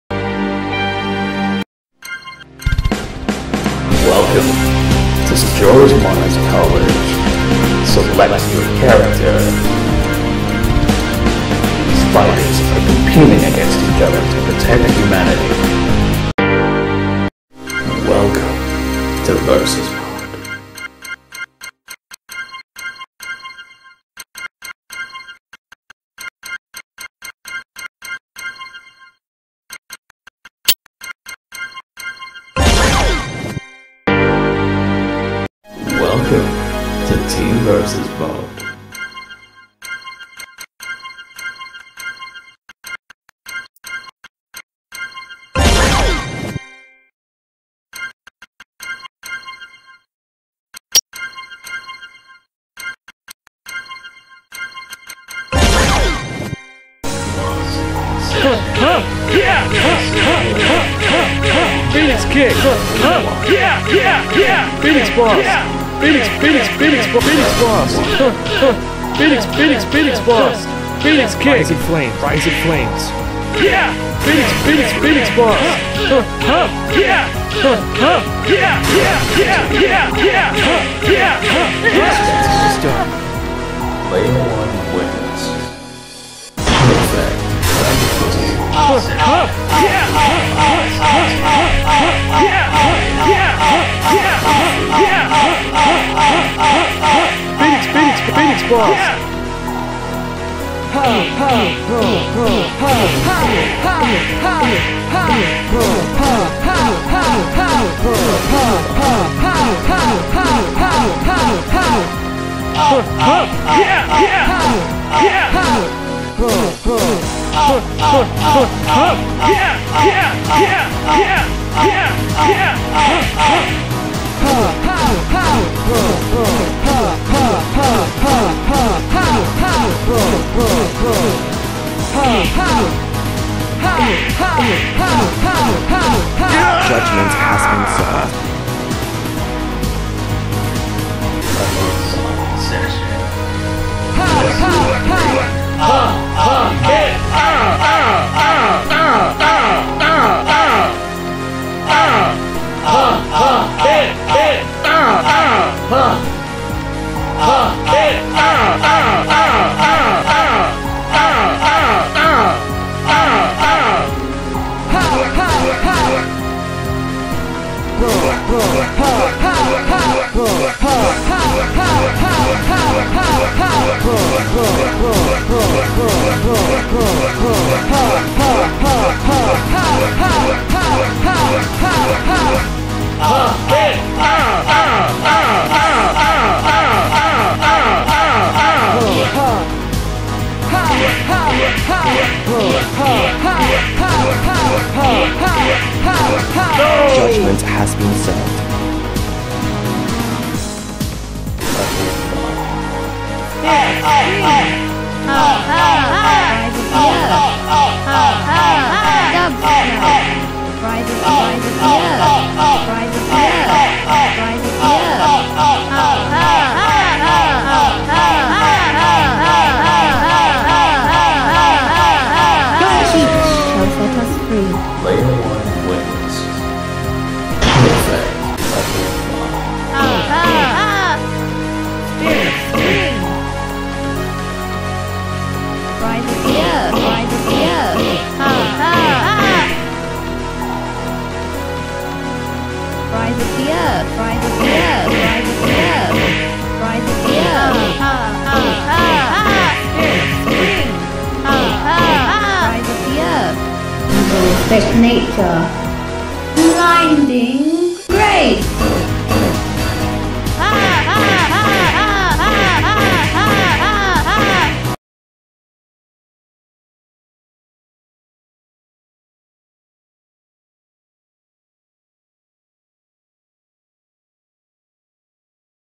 Character Select Name Plates + Team VS Mode Update
Just adding some small touches to the presentation of the game now. I still need to finish Banika and Evil Rubel off lol. xD
Also I need to add more round animations as well. The last two rounds look really odd..